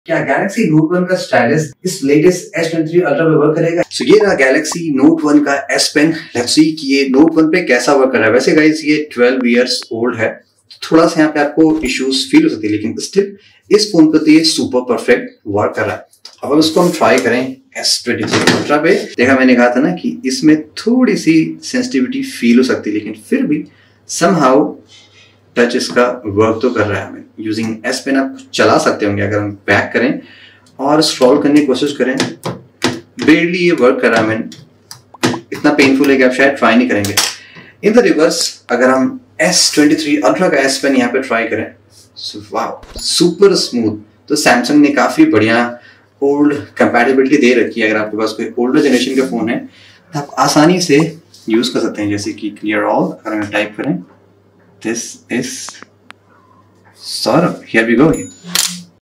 Samsung Galaxy Note 1 vs Samsung Galaxy S23 Ultra
Trying the Stylus or S-Pen of 1st Samsung Galaxy Note 1 on latest Samsung Galaxy S23 Ultra ! Lets find out whether it is working or not.
See our Host Saurabh on his social network,